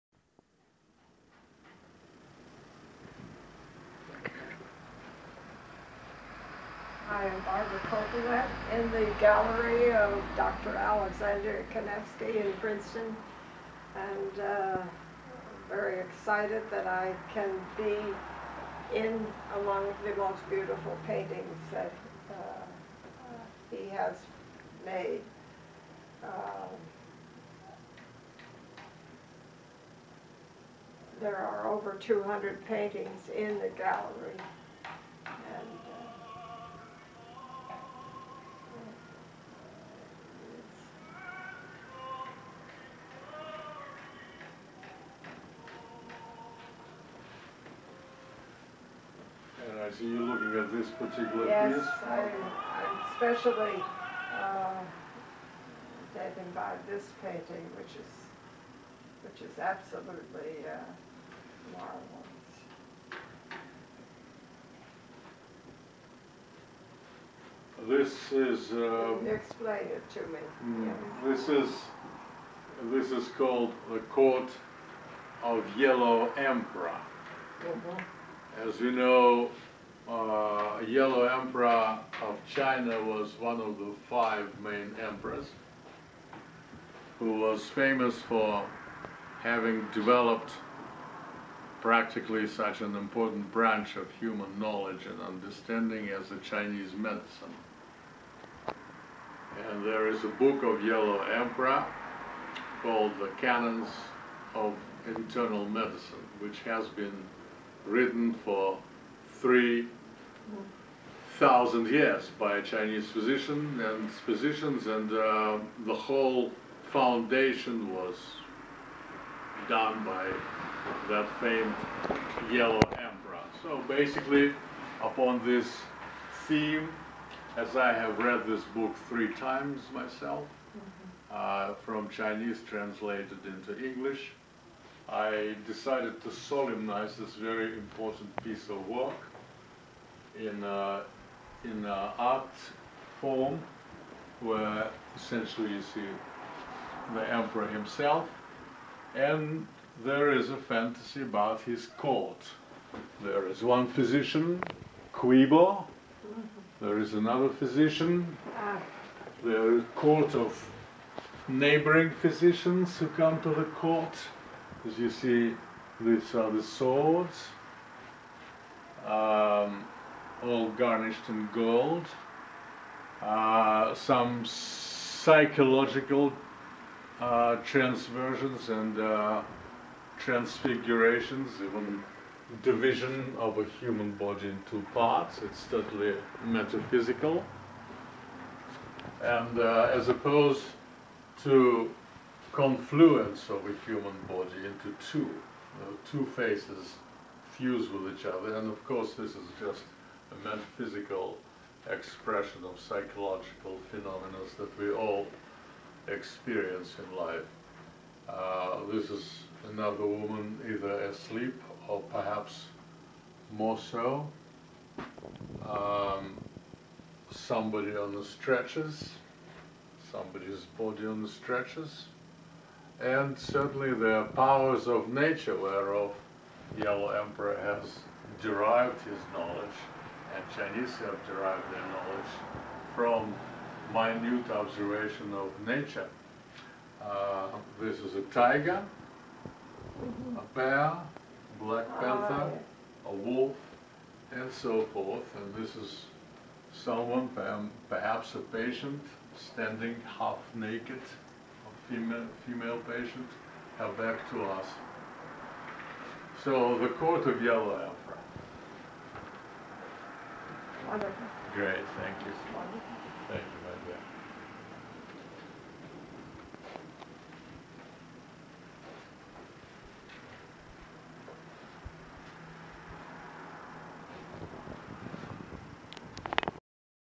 Alexander Kanevsky and collector discuss Alexander's "Yellow Emperor".
Besides the marvel of the painting the collector Barbara Cokelet was happy to learn the story of the "Yellow Emperor" who has developed the Science of Chinese Medicine. The painting is located in Princeton  Kanevsky Gallery of Art which is a part of World Ambassador of Peace Sacred Prince N:.H:. Monsignor Sandro Pulin's "Art for Peace in the World" organization.